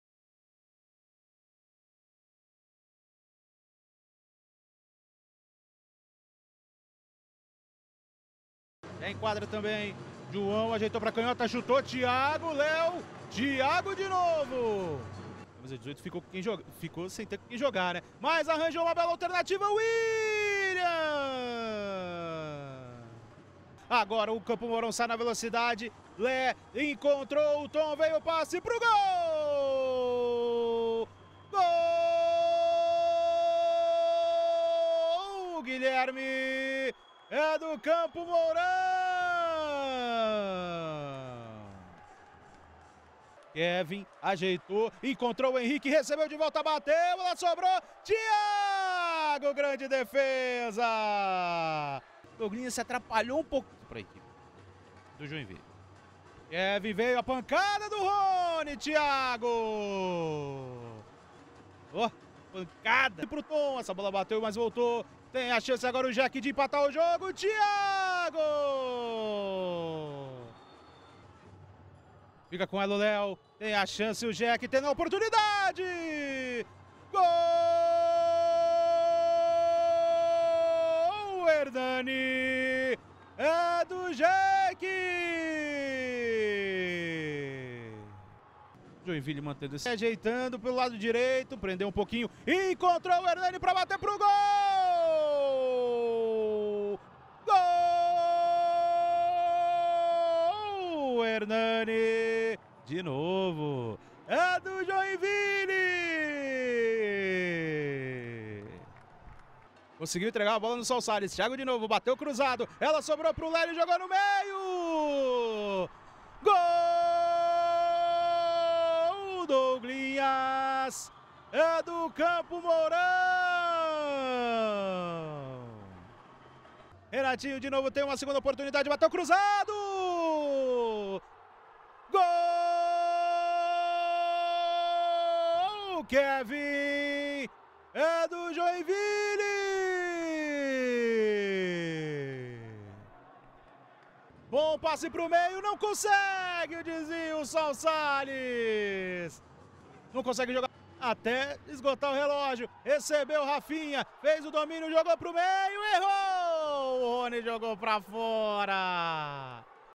LNF MRJACK.BET 2023 - 5ª RODADA - GOLS - MOU 2 X 3 JEC
GOLS:
CAMPO MOURÃO - GUILHERME E DOUGLINHAS
JOINVILLE - ERNANI (2) E KEVIN
TRANSMISSÃO: LNFTV